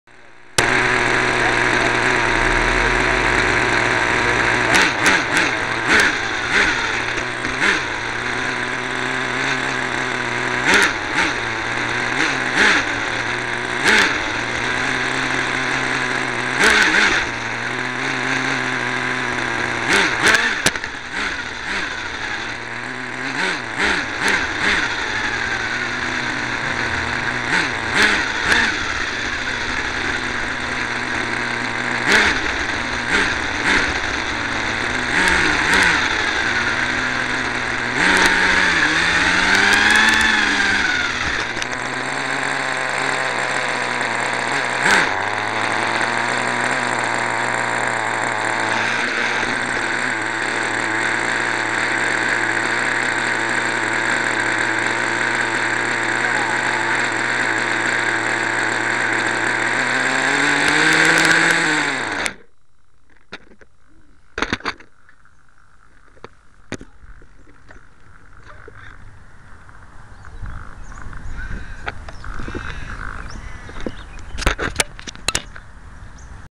RC Nitro Car with first try mini camera
Neighbours car, my camera. Doesn't go far before hitting gutter.